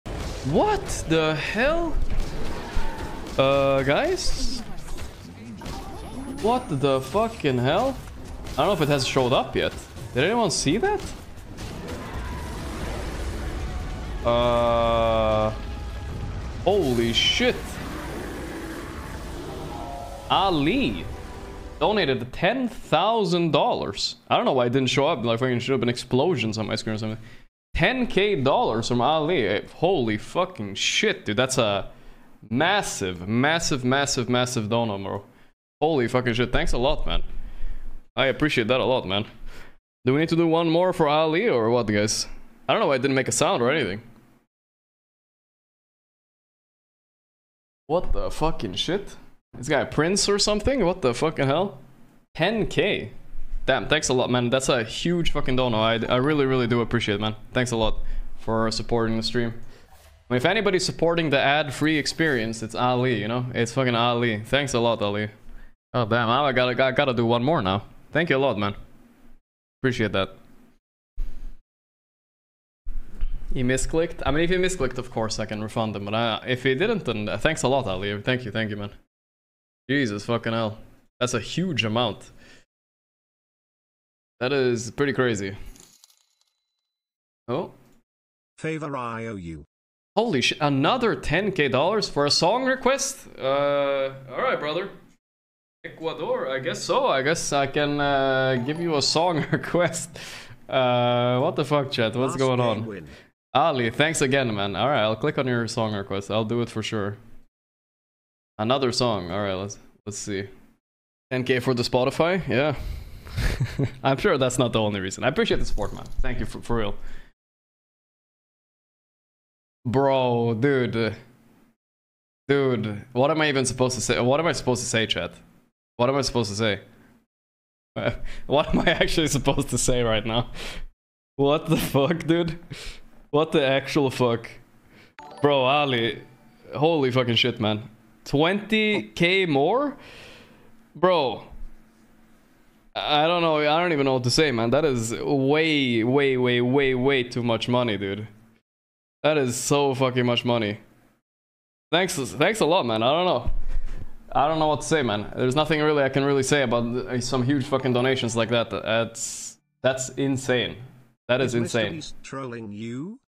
Gorgc & chat reaction to $40,000 total dono by Ali GIGACHAD
Credits:
Gorgc